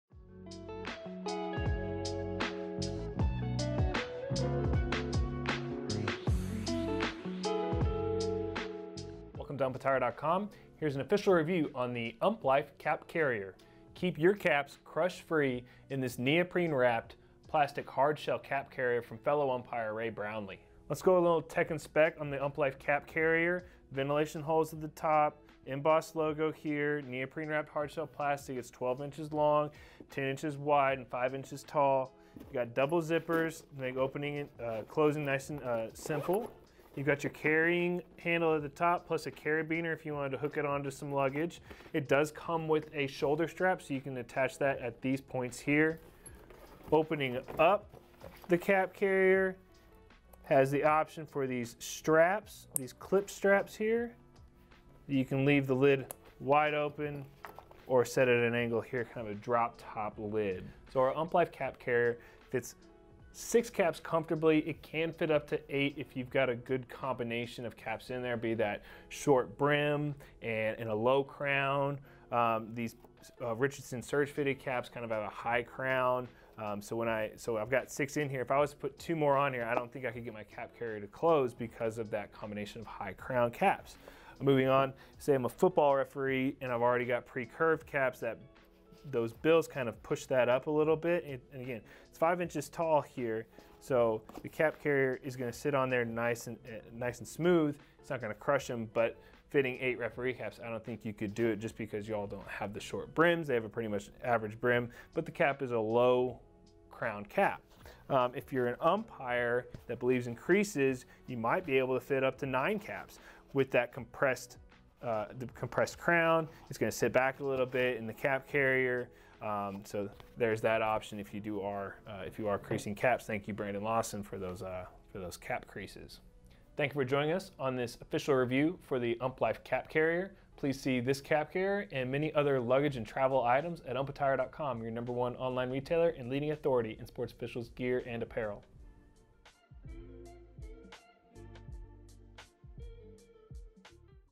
Official Review: UMPLIFE Cap Carrier
If you have multiple umpire or referee caps, you can now store and protect them all in this convenient hard-shell carrying case. The UMPLIFE Cap Carrier holds any number of caps up to 8.